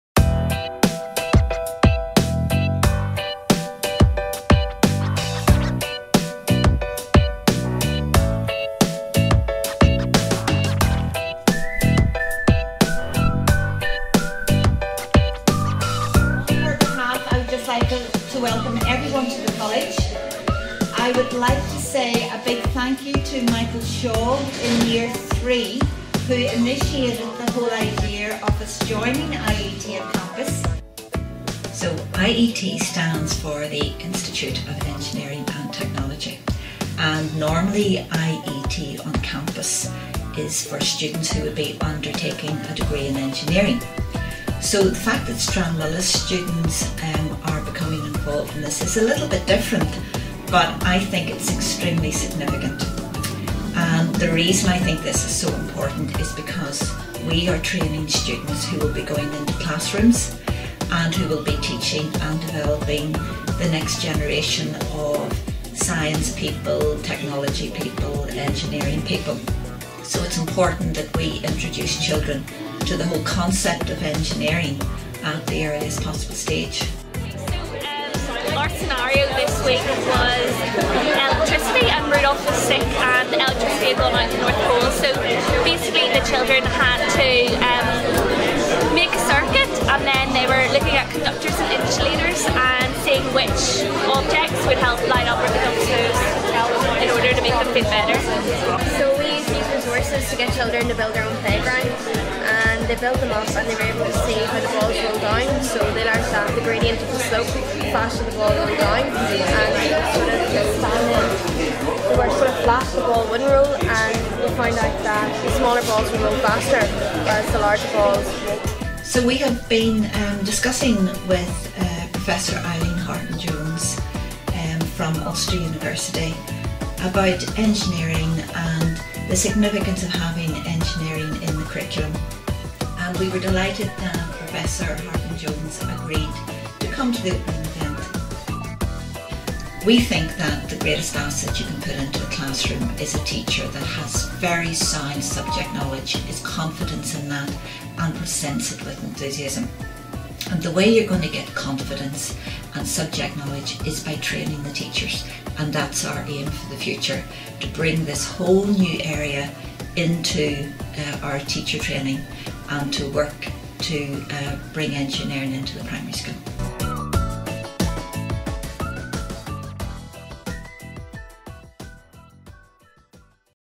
Launching IET on Campus at Stranmillis
In December, our Head of Mathematics, Science and Technology, Dr Irene Bell, and Professor Eileen Harkin-Jones from Ulster University launched the IET on Campus initiative at Stranmillis University College, at an event which showcased some of the work and ideas of students teaching engineering at primary school level.

In this video, Dr Bell explains what IET on Campus is, and why it's a significant development for Stranmillis University College.